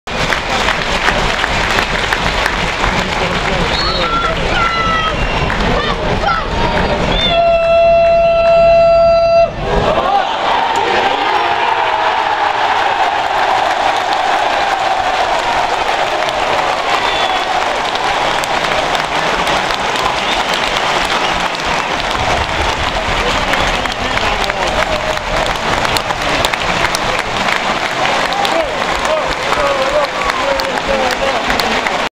Jose Fonte's goal againest Hartlepool 2011
Rickie Lamberts free kick to Jose Fonte at the back post who heads it in the back of the net!!